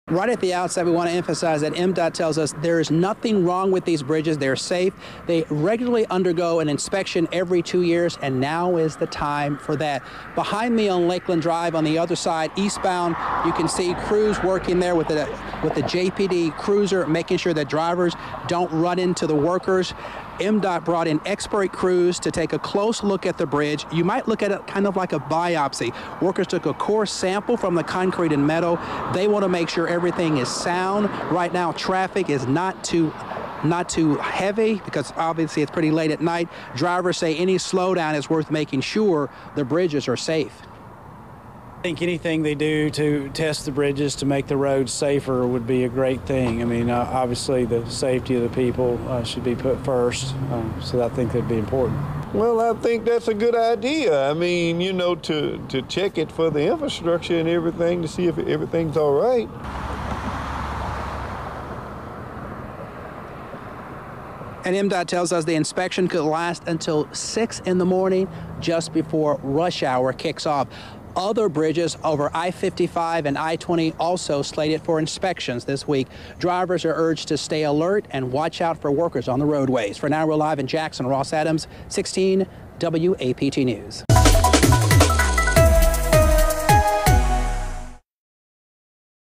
Crews inspecting bridges, state roads and interstates.
Late night traffic alerts. Crews are inspecting bridges, state roads and interstates.